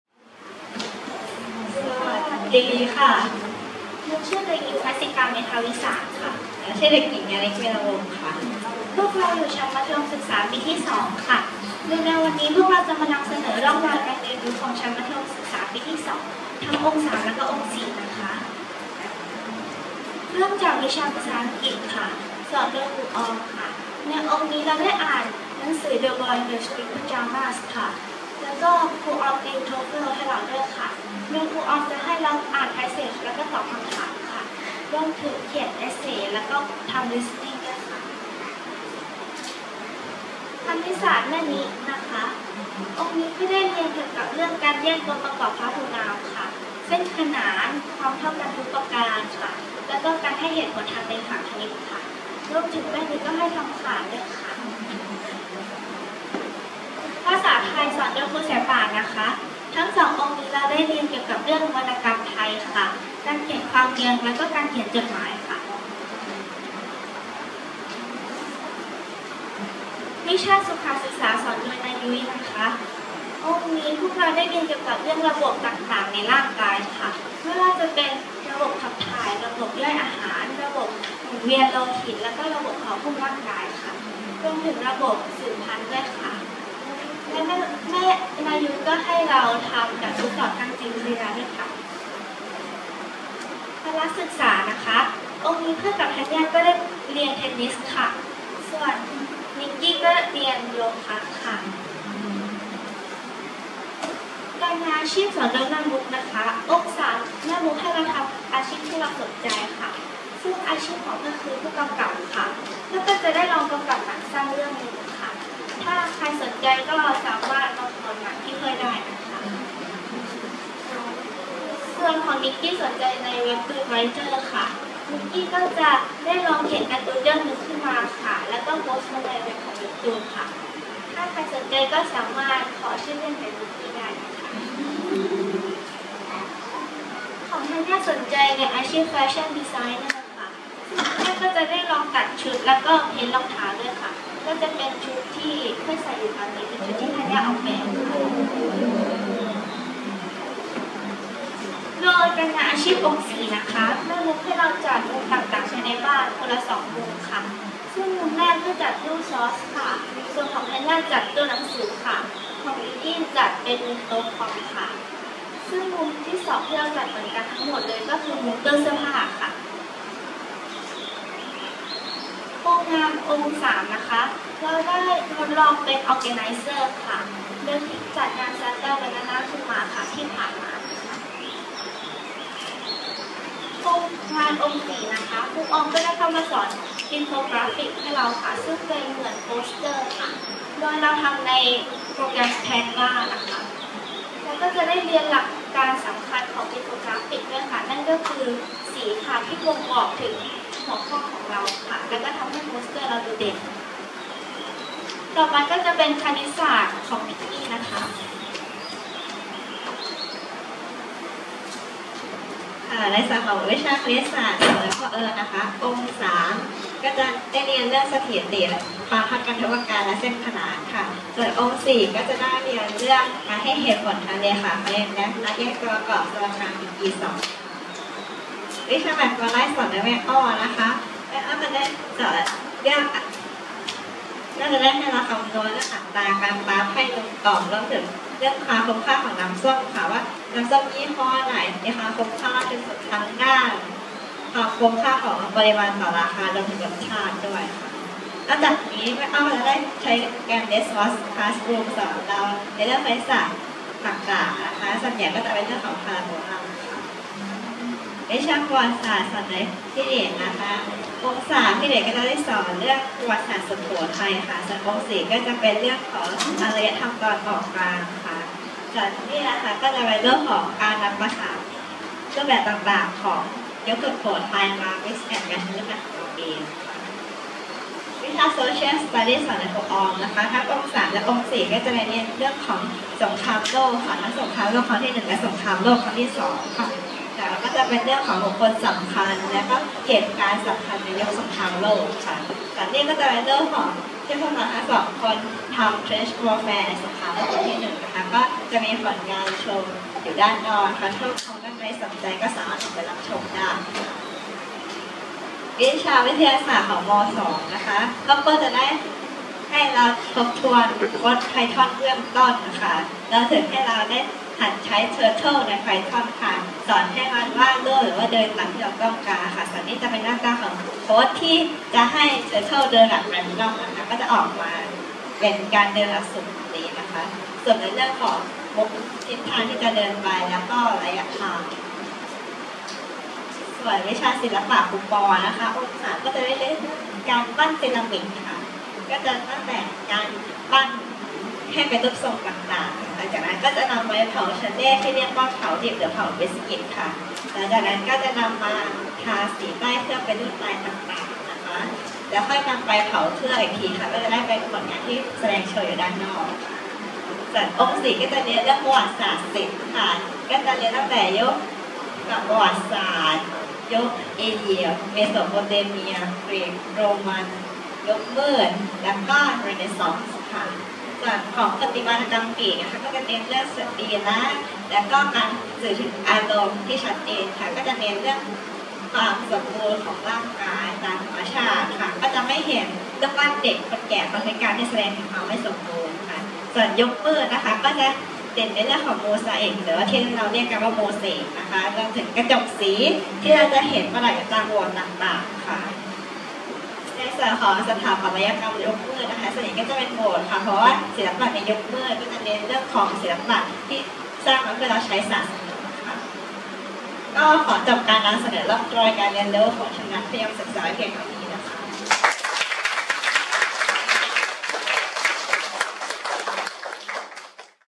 ร่องรอยการเรียนรู้ม.2 2021-04-02
งานปิดองค์ 4 ที่ศูนย์การเรียนปฐมธรรม (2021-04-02)

มีผลงานและร่องรอยการเรียนรู้ต่างๆของเด็ก มีการแสดงดนตรีไทย ดนตรีสากล และละครภาษาไทย ภาษาอังกฤษครับ